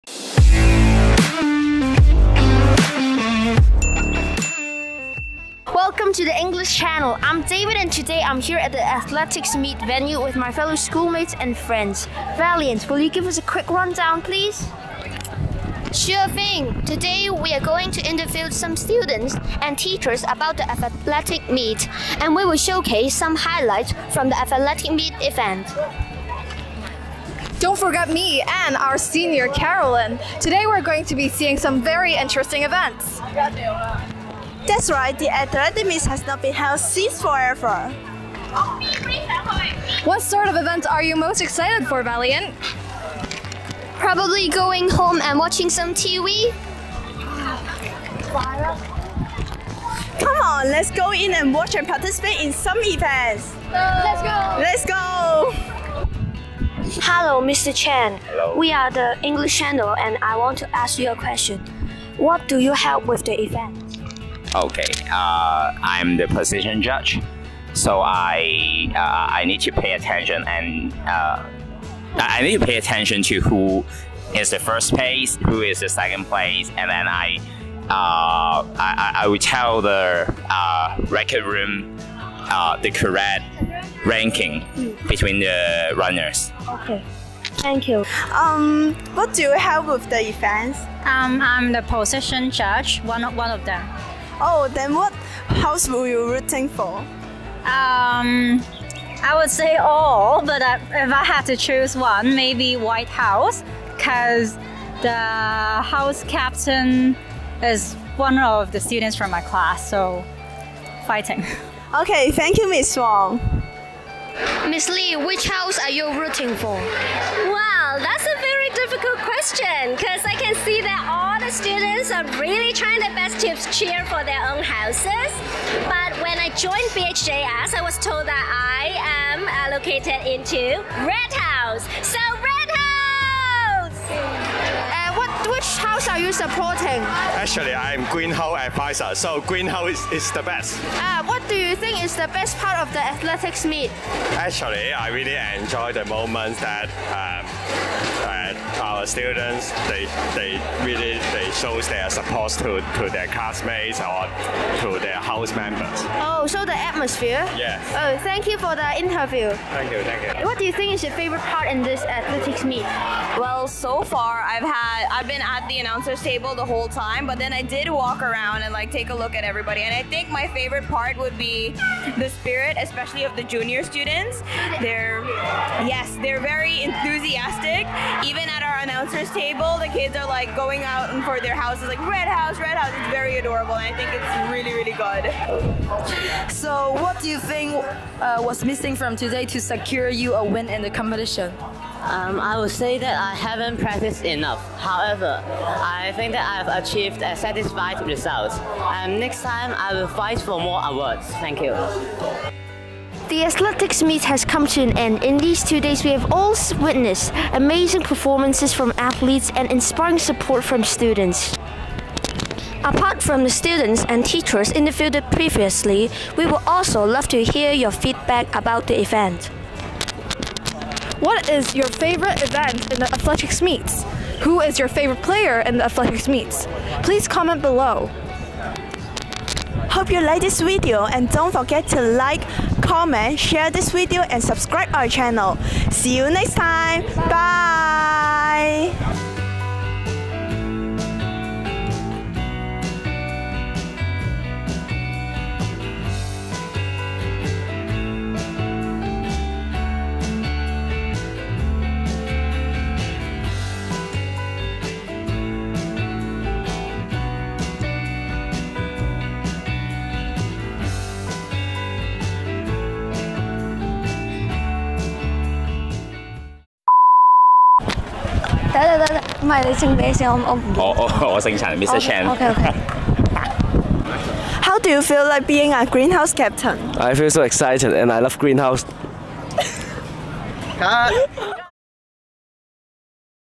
BHJS Athletics Meet
After a 4-year pandemic-induced break, our Athletics Meet is back! Brace yourself for incredible displays of athleticism and determination. Watch as our athletes make history with their speed, strength, and unwavering spirit. Don't miss out on this epic event! 🏃🎉